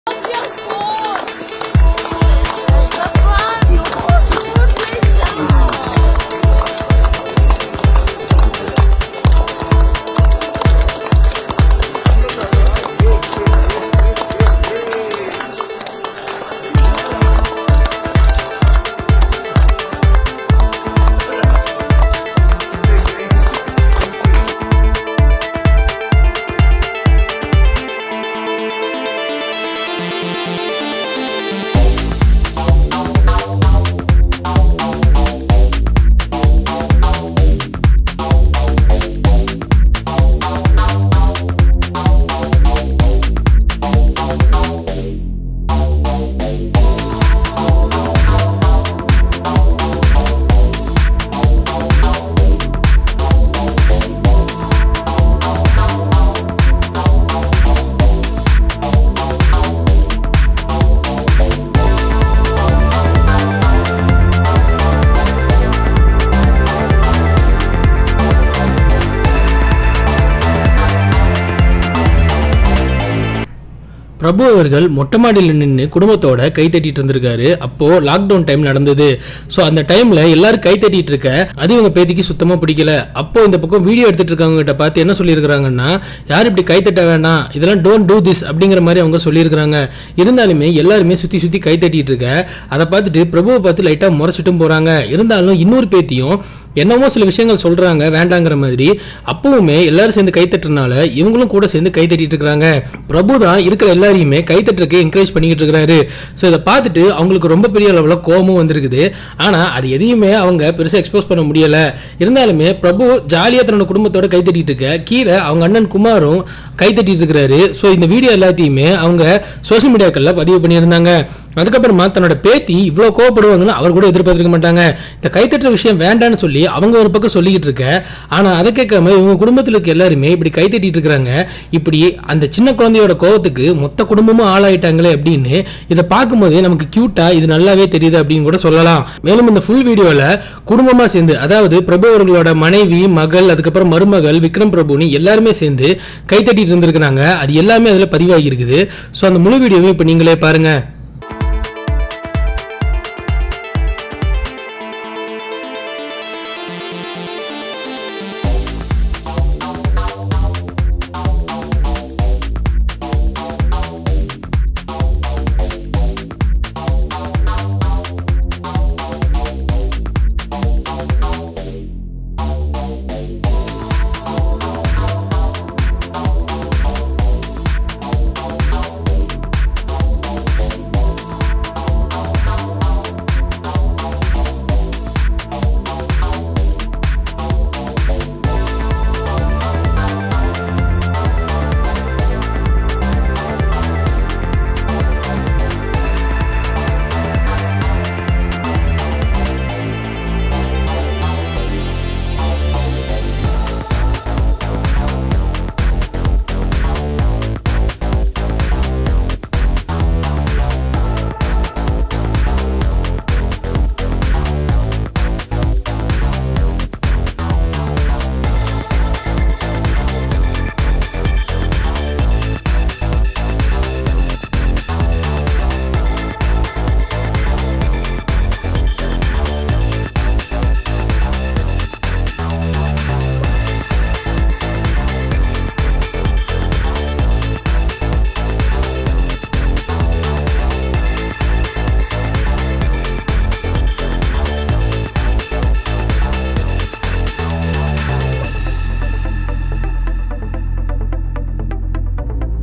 🔴 VIDEO: மெட்ட மாடில பிரபு பேத்தி கடுப்புல பண்ணத பாருங்க! Prabhu | Vikram Prabhu | granddaughter
Prabhu house video!

Prabhu is a very famous actor in tamil cinema. In this video we are going to see Prabhu house video.